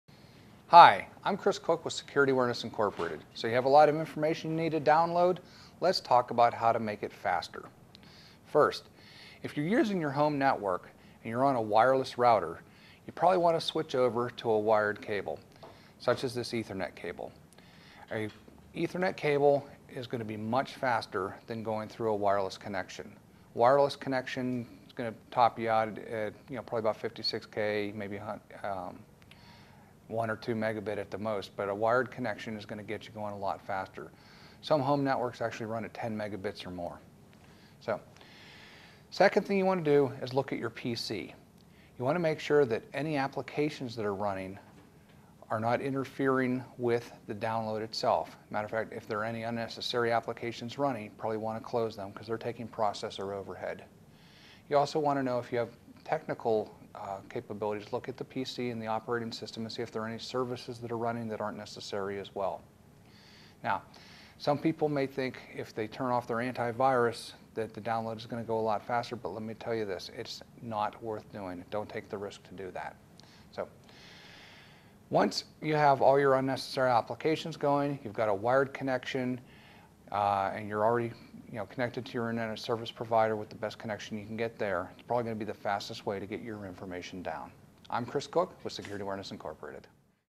Basic Internet & E-mail Skills : How to Make Downloading Faster
Making downloading faster on your home computer starts with getting a broadband Ethernet connection and checking for interfering PC applications. Surf the Internet and download songs or data faster by setting up an Ethernet connection with tips from a computer specialist in this free video tutorial.

Bio: Chris Cook has been CEO of Security Awareness Inc. for more than nine years.